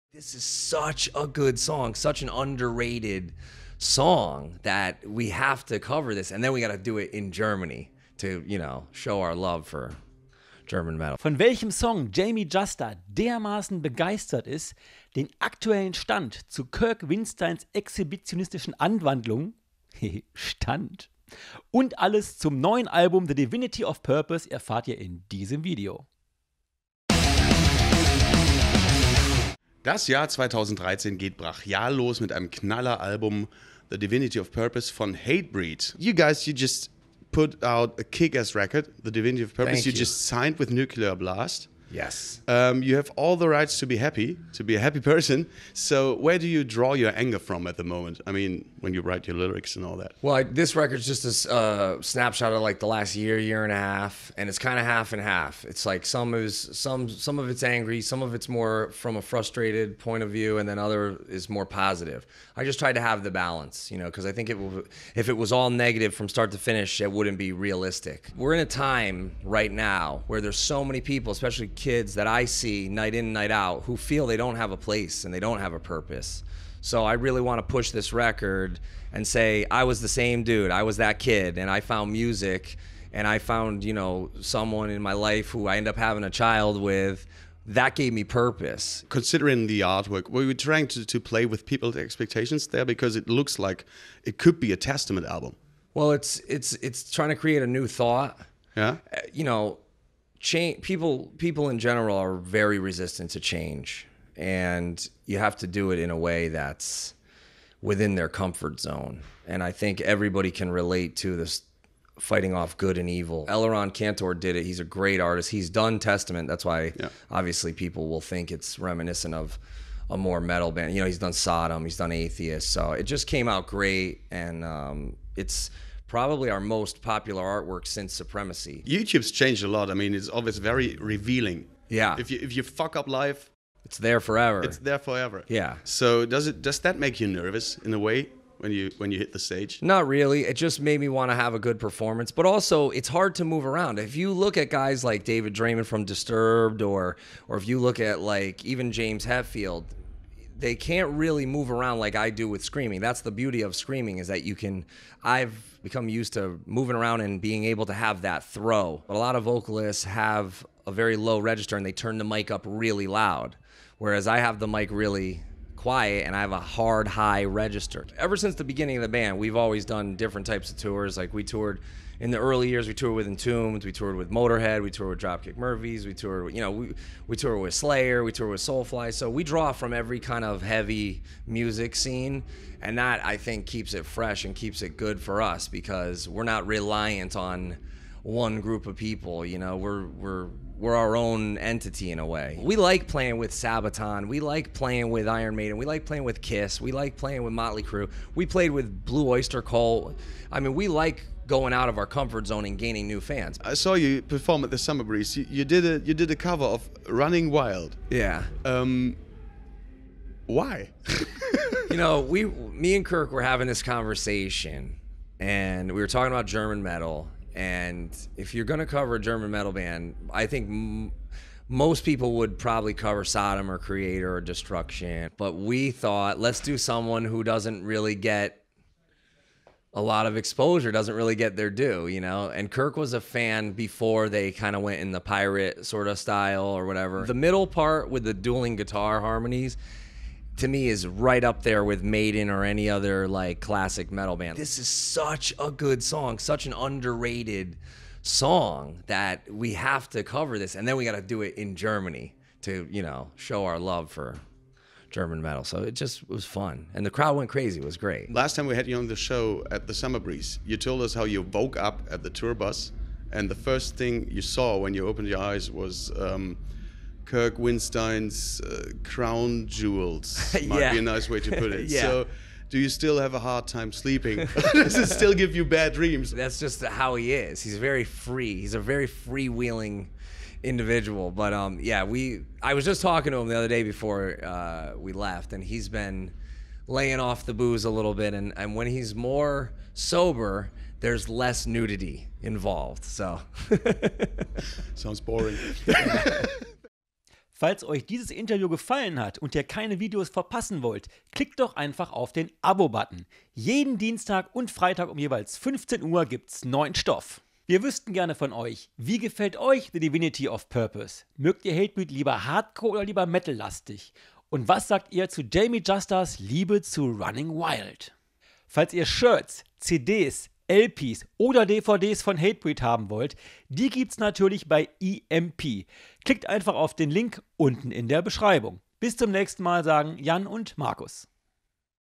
HATEBREED - Der Sinn des Lebens
Jamey Jasta spricht ausführlich über Musik und Texte des neuen Albums sowie seinen Respekt für Running Wild.